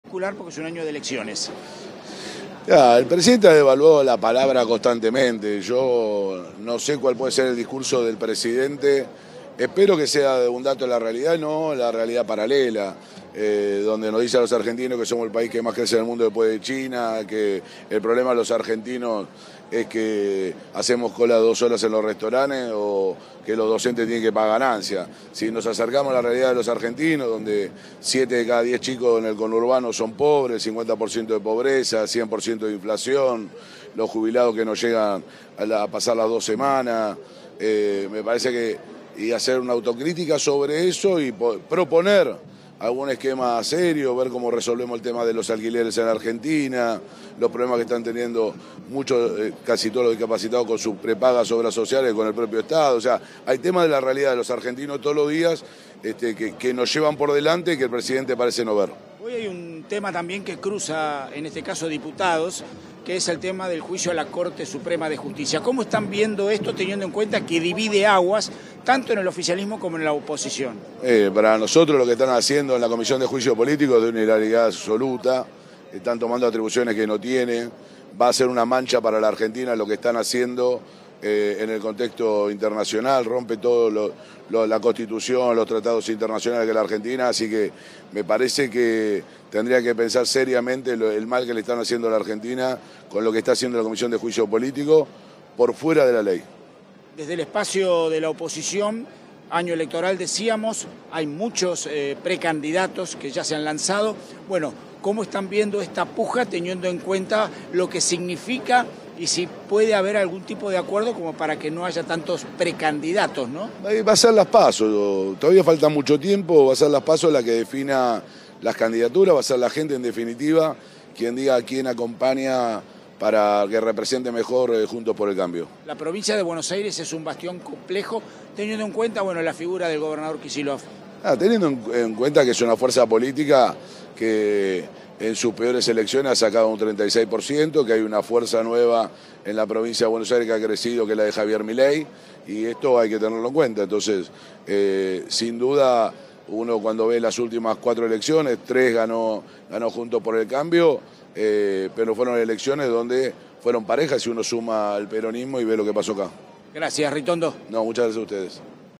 Ritondo: "El Presidente ha devaluado la palabra constantemente"
El diputado de Juntos por el Cambio Cristian Ritondo criticó al presidente Alberto Fernández en el marco del inicio de la Asamblea Legislativa 2023. "Espero que el discurso del Presidentes sea con datos de la realidad y no de una realidad paralela", expresó.